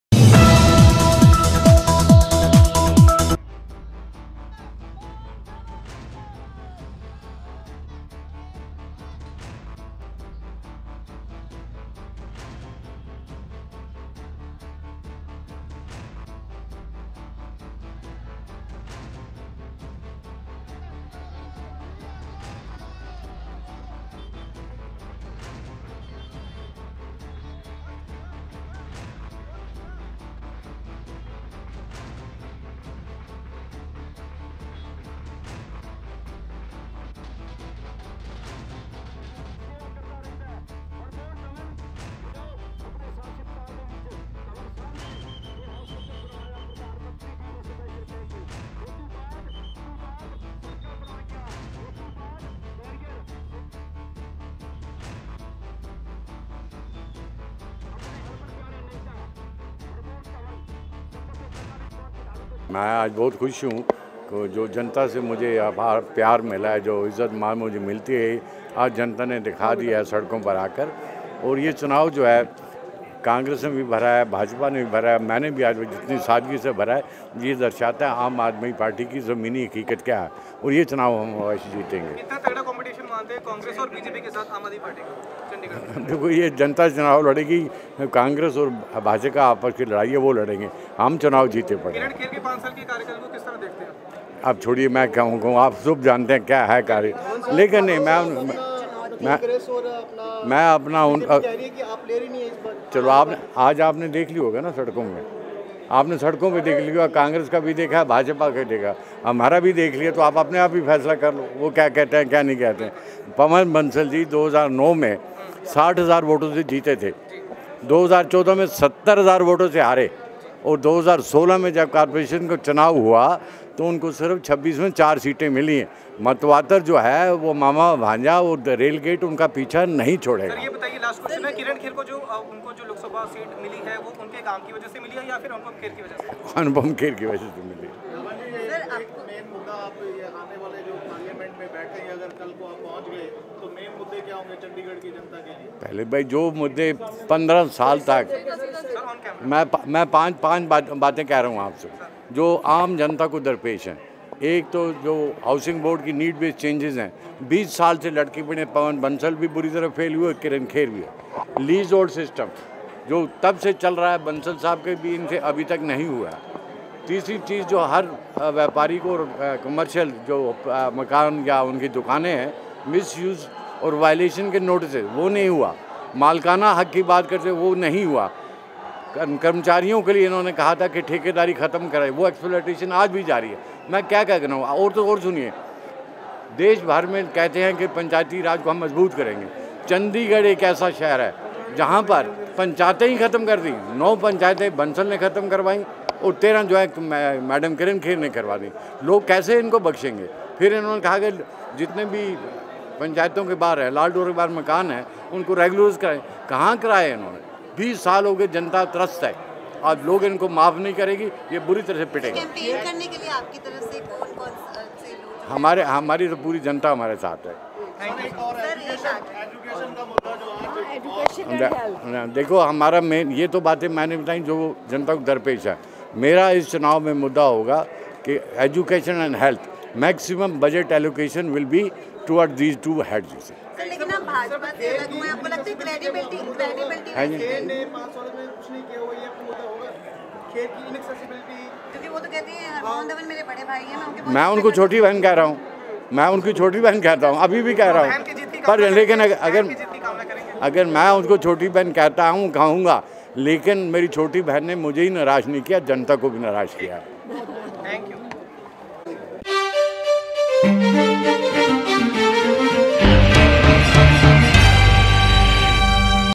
नामांकनपत्र दाखिल करने के बाद हरमोहन धवन बोले - ये बुरी तरह पिटेंगे !
नामांकनपत्र दाखिल करने के बाद Harmohan Dhawan बोले - ये बुरी तरह पिटेंगे !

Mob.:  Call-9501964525